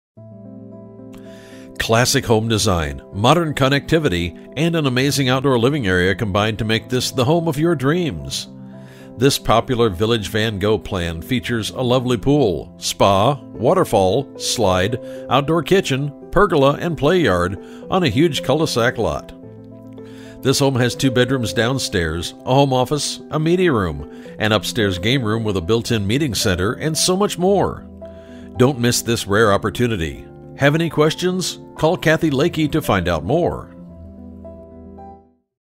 1801 Graystone Hills Court Conroe, TX | ColdwellBankerHomes.com
1801 Graystone Hills Court, Conroe, TX

Classic home design, modern connectivity, and an amazing outdoor living area combine to make this the home of your dreams! Popular Village Van Gogh plan features pool, spa, waterfall, slide, outdoor kitchen, pergola, and play yard with rubber mulch added in 2019. Pool not only has a heater, but it h